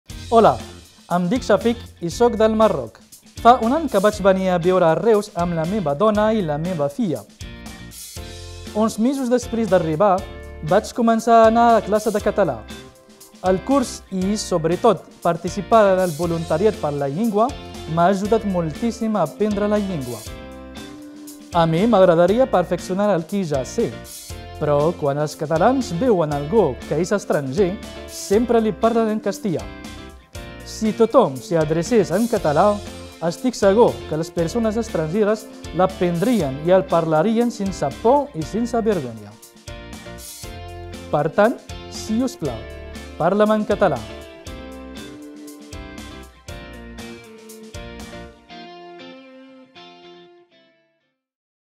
Parla'm en català: Chafik
La campanya “Parla’m en català”, impulsada pel CNL de l’Àrea de Reus, pretén convèncer la població catalanoparlant de parlar en català als nouvinguts, ​i també vol animar els qui l’estan aprenent a demanar que se'ls parli en català.